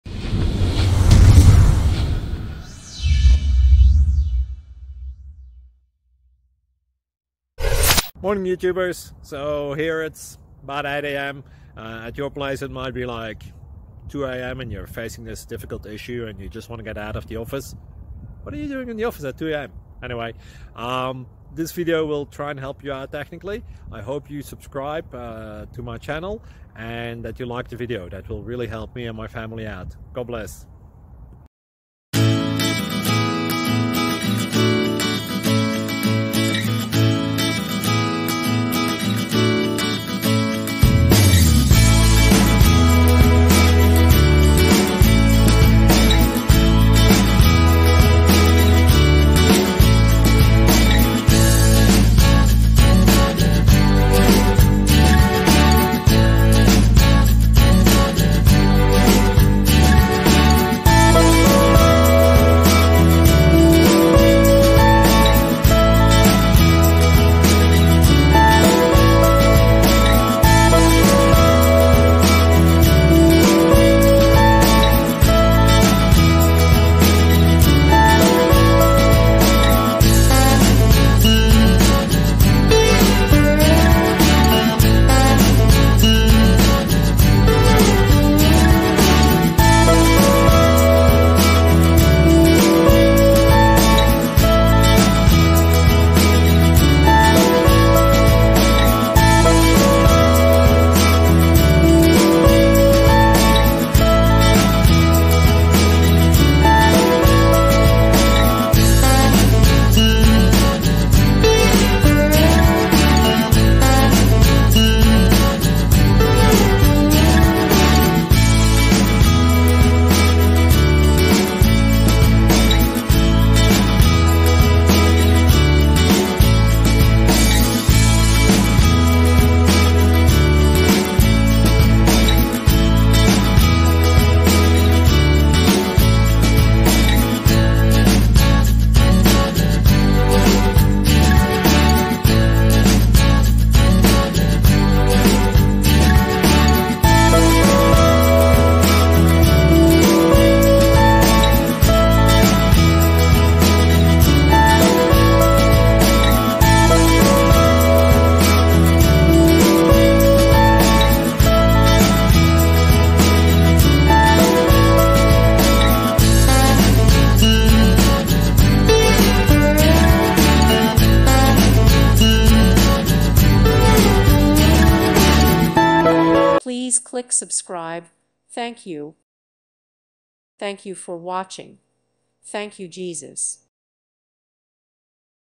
Efficient techniques to make a clean rename of a Ubiquitous Language term (DDD) throughout codebase
With thanks & praise to God, and with thanks to the many people who have made this project possible! You are responsible for your own actions.